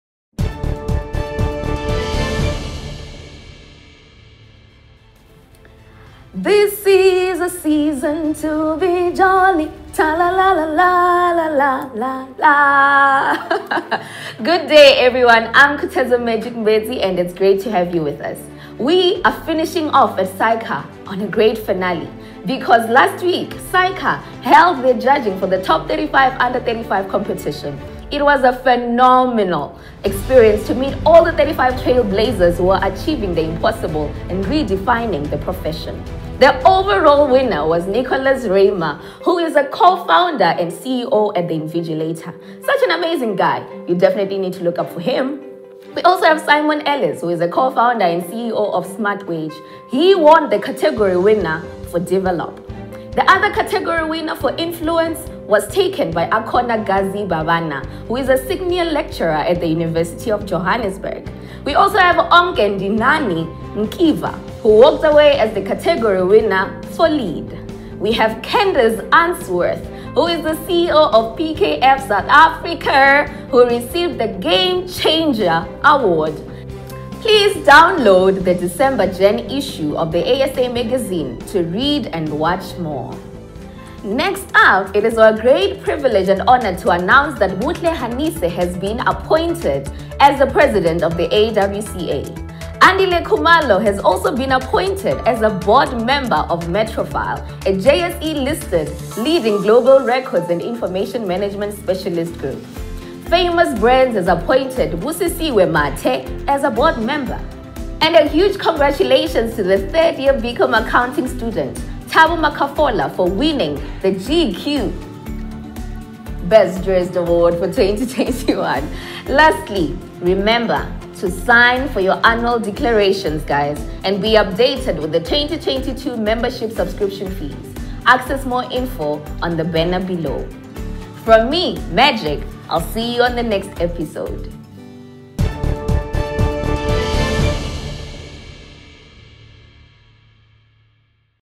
Episode1: Congratulations to the Top 35-under-35 winner & category winners & new member appointments
Episode 1: Congratulations to the Top 35-under-35 winner and category winners and new member appointments.